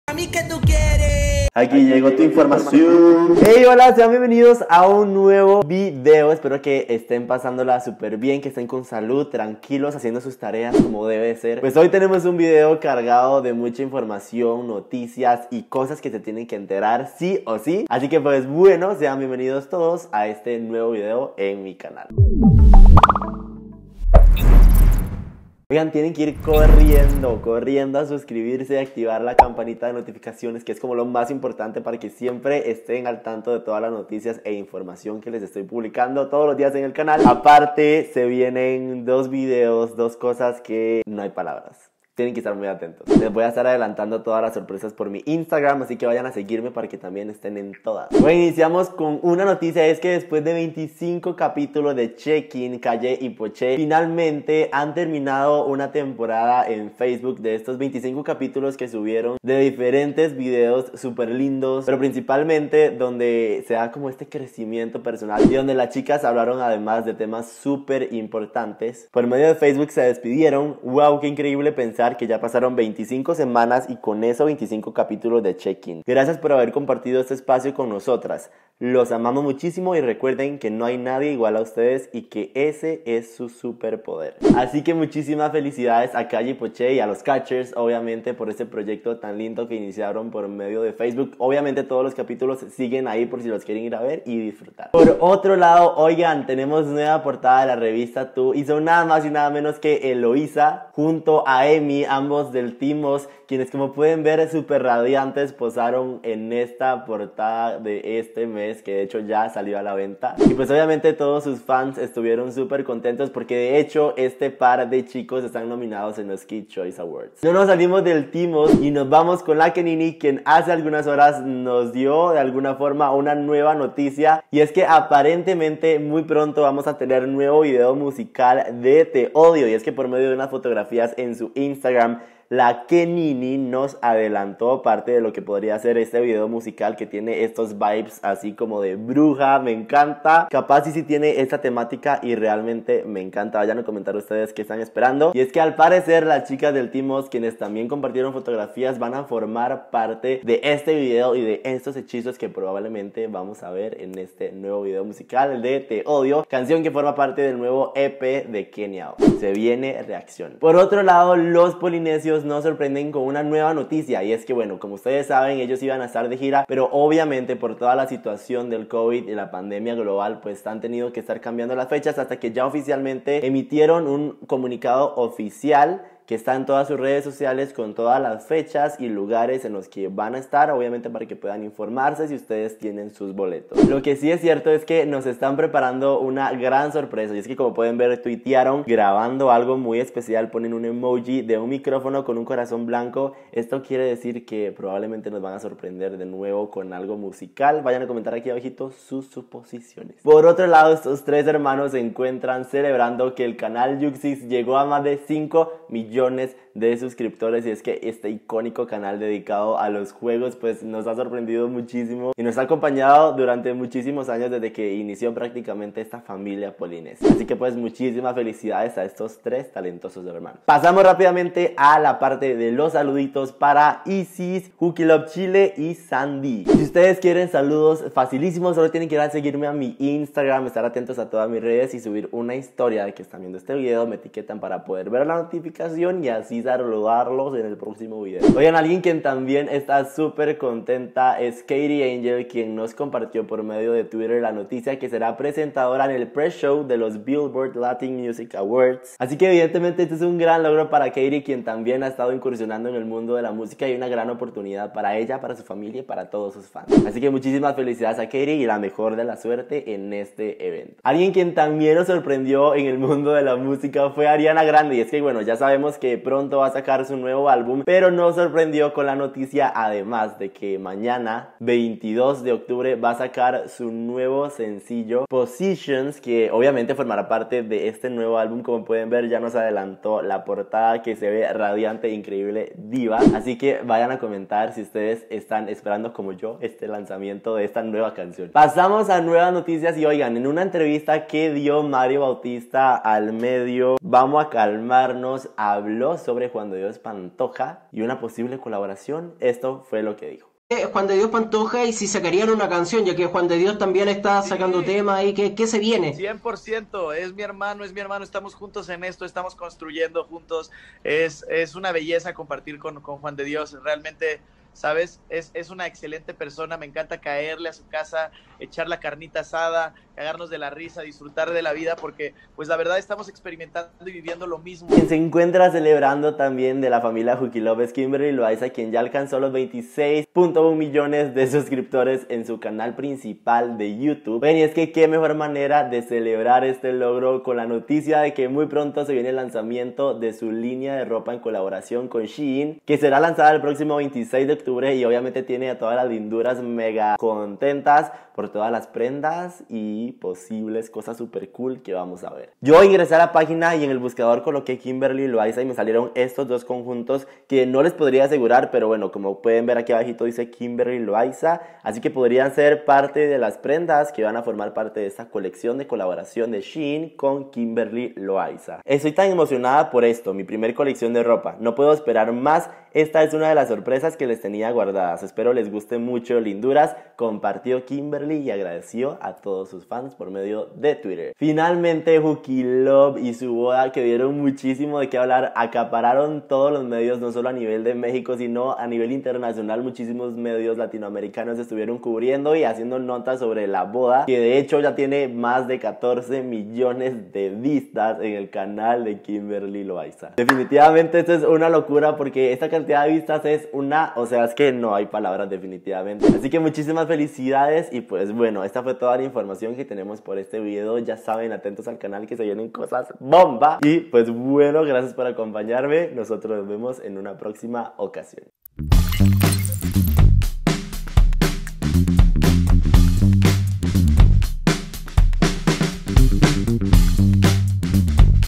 LÍNEA DE ROPA DE KIMBERLY LOAIZA, LOS POLINESIOS PREPARAN SORPRESA, KENIA OS DA UN ADELANTO
✨ Espero que les guste este video y hayan quedado super informados❤️